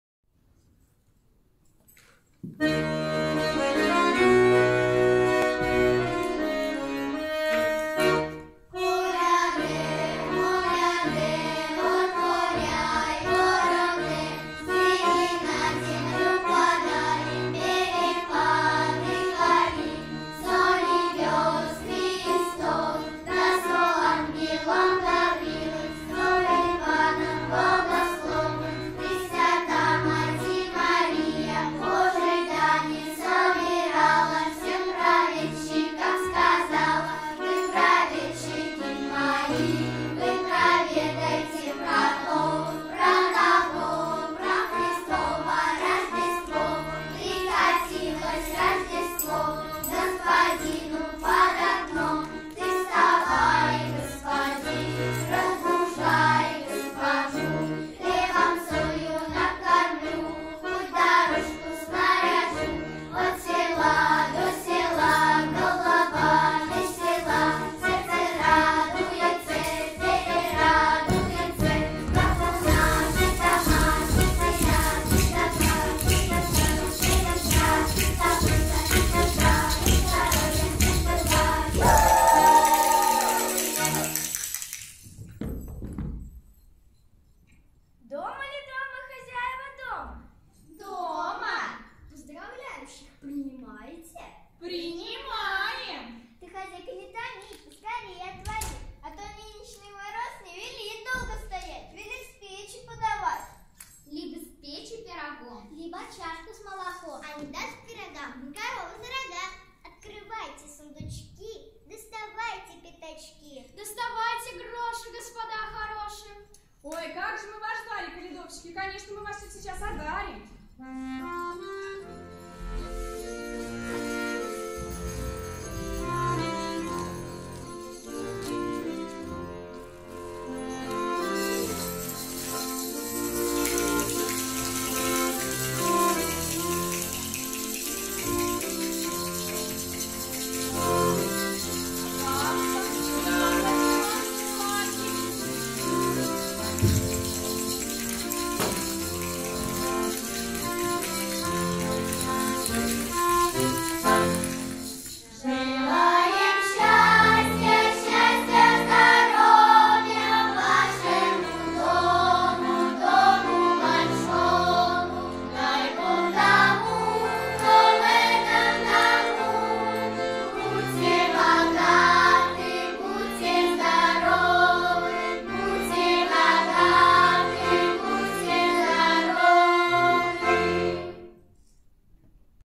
Фольклорный ансамбль «Сударь и сударушки»
Фольклорный ансамбль «Сударь и сударушки» приглашает погрузиться в атмосферу яркого праздника рождественских колядок! Колядки - это веселье, дружба, положительные эмоции и добрые воспоминания о празднике. А ещё повод провести день в кругу друзей, помириться с теми, с кем в ссоре. Это повод пожелать всем счастья, добра, взаимопонимания и хорошего настроения!

Преподаватель Толстикова Анжелика Валерьевна
 Концертмейстер Чумаков Игорь Витальевич